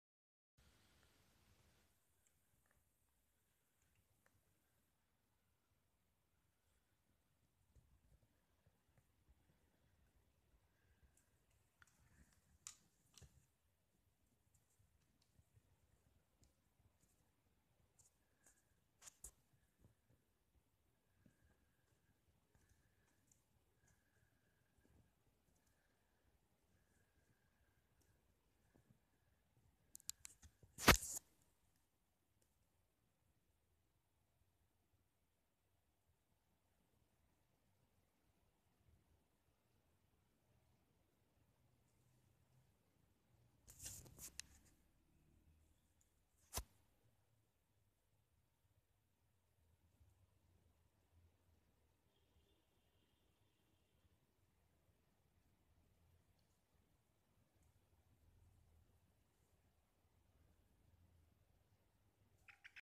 Chipmunk Karachi Pakistan
His name is Tutu, I found him he was a week old. Now an year old he lives freely with his friends and family.